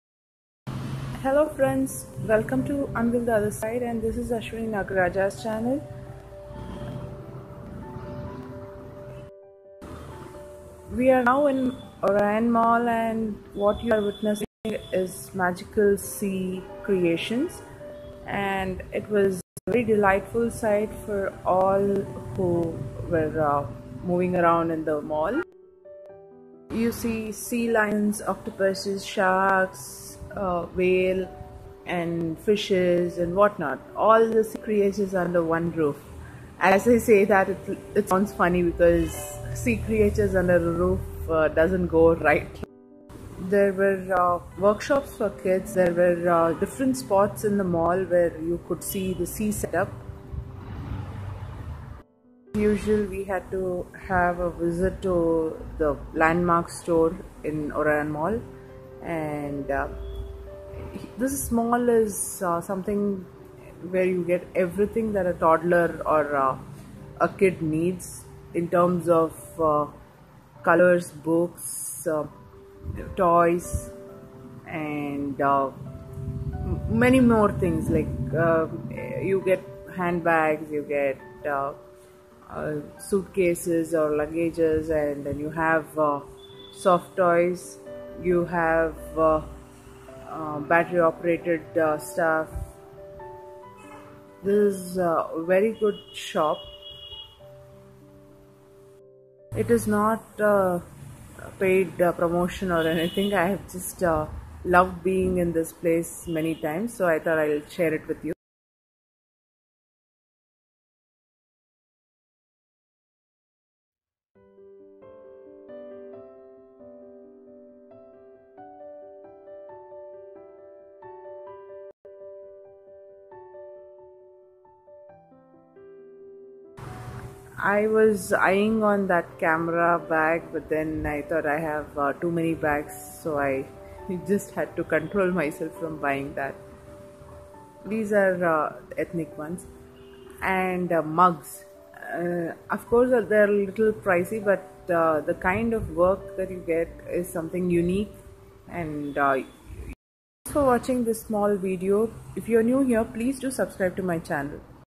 Magical sea in Orion mall, and a small glimpse of Landmark shop.
While we were at Orion mall a few days ago it was a wonderful sight for us with all the sea stuff around us. We had a very good time.. Thought of sharing a few glimpses with you guys as well. Hope you'd love it.